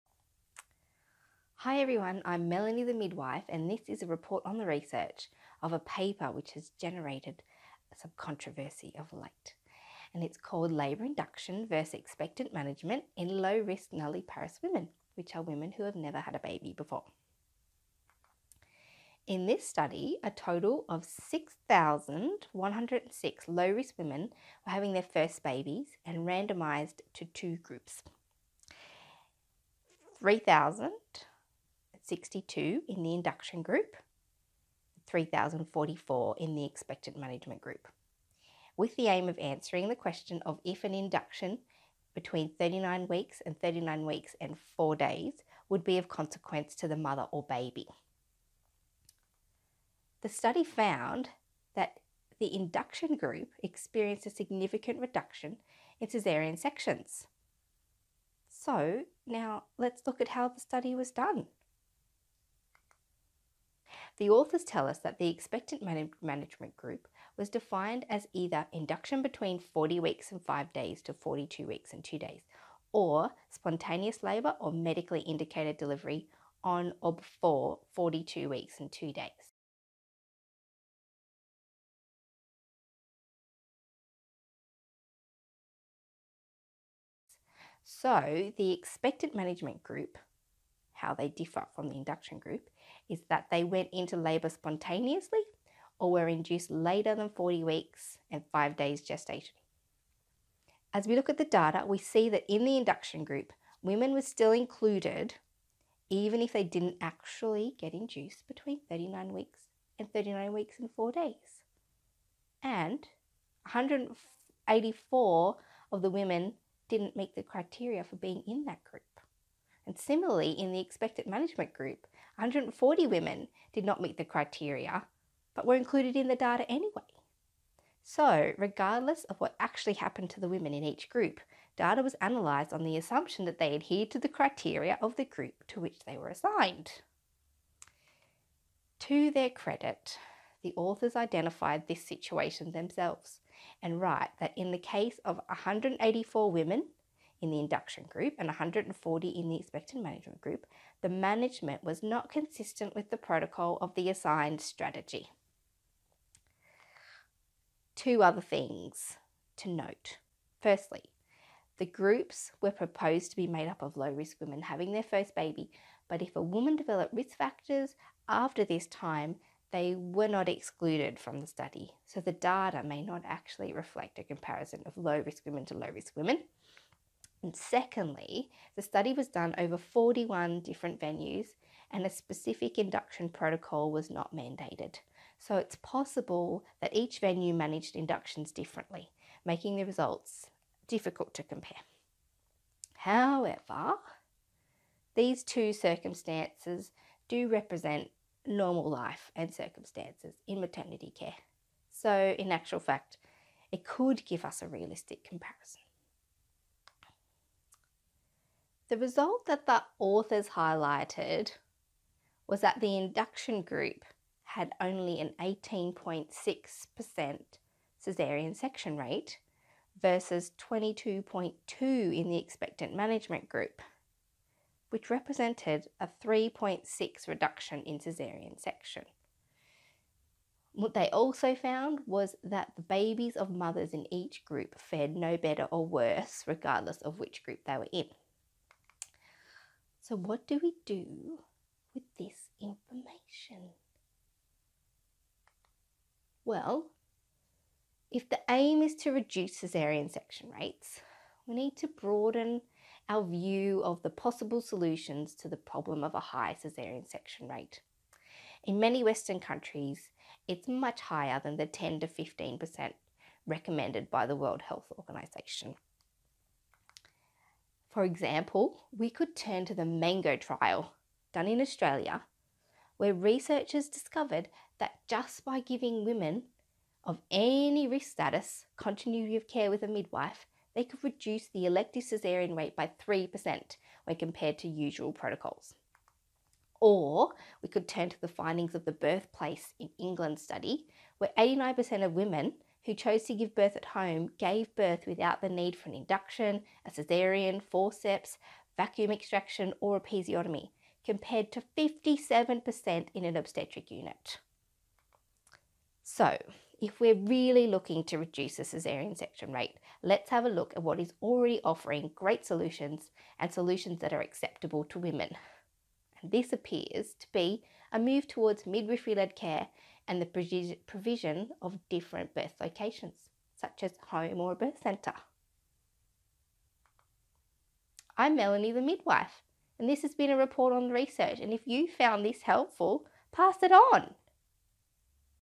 Labor induction vs expectant management in low risk nulliparous women
In this video, Melanie the Midwife quickly summarises and reviews the article "Labor induction vs expectant management in low-risk nulliparous women" written by William Grobman and team published in the New England Journal of Medicine 9/8/18.